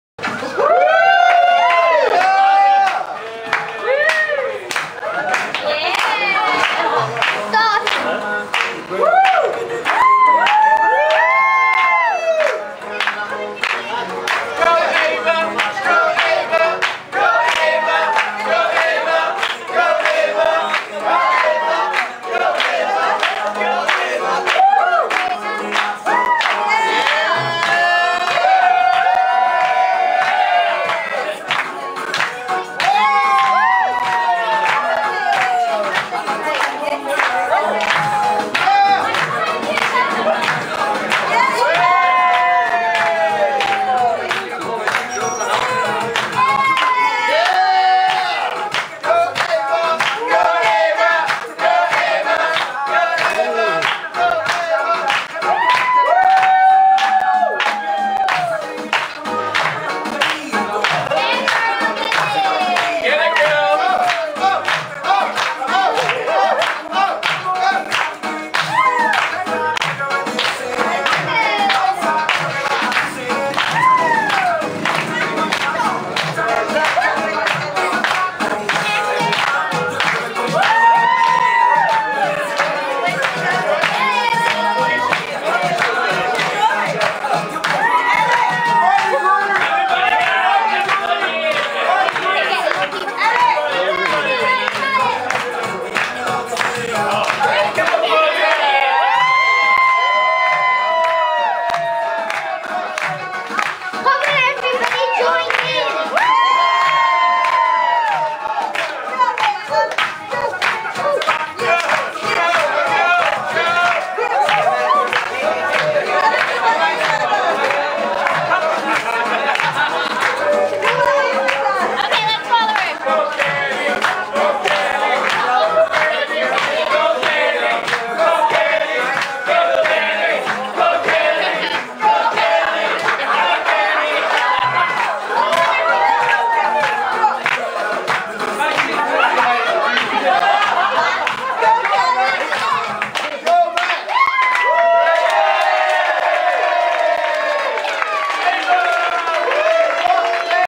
Ava dances!
4 year old Ava decided to get the party started at the wedding reception at Paros Greece